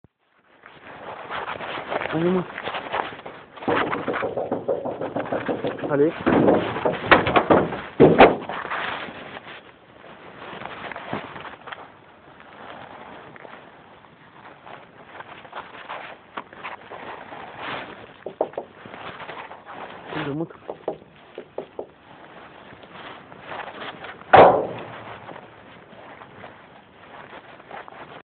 !! Chien de citée qui detruit une voiture en direct !!
ce chien est capable de tout :o